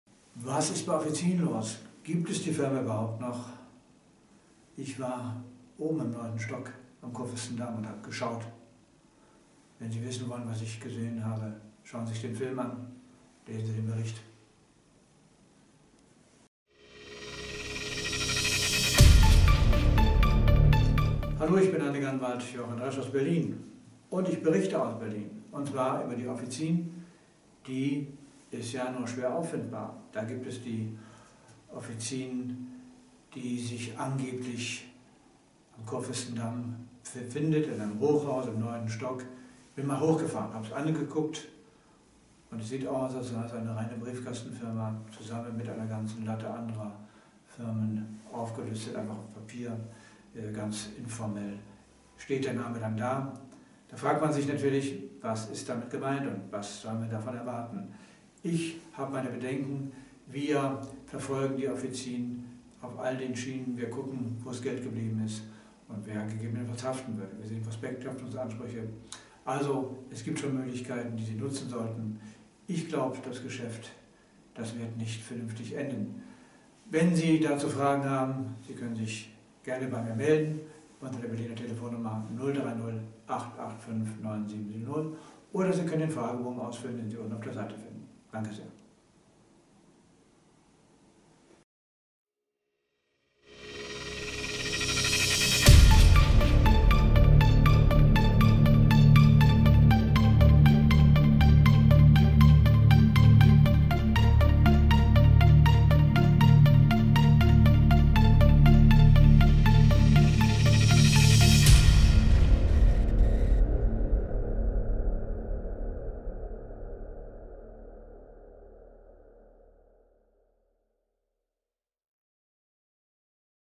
Gibt es Offizin noch?
Anlegeranwalt Jochen Resch war vor Ort auf der Suche nach der Offizin. Schauen Sie, was er gefunden hat.

Unser Standort:
Resch Rechtsanwälte
Ernst-Reuter-Platz 8
10587 Berlin